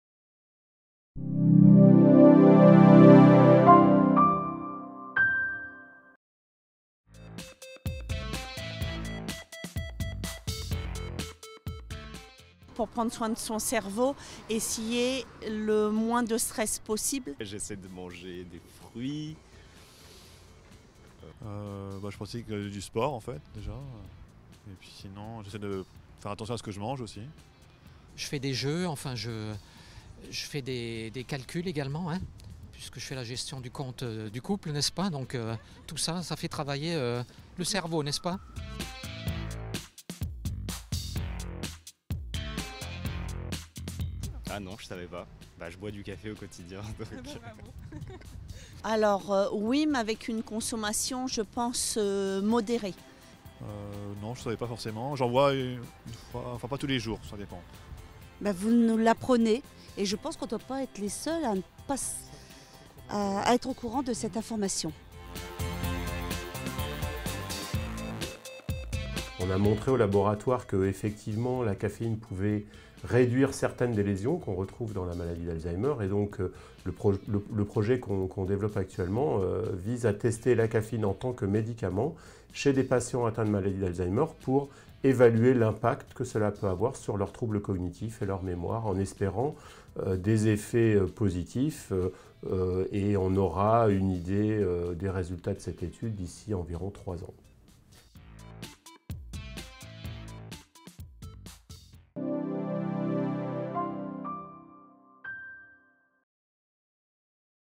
Une astuce pour entretenir votre cerveau !
Que faites-vous pour entretenir votre cerveau au quotidien?...Nous vous révélons une astuce accessible à tous.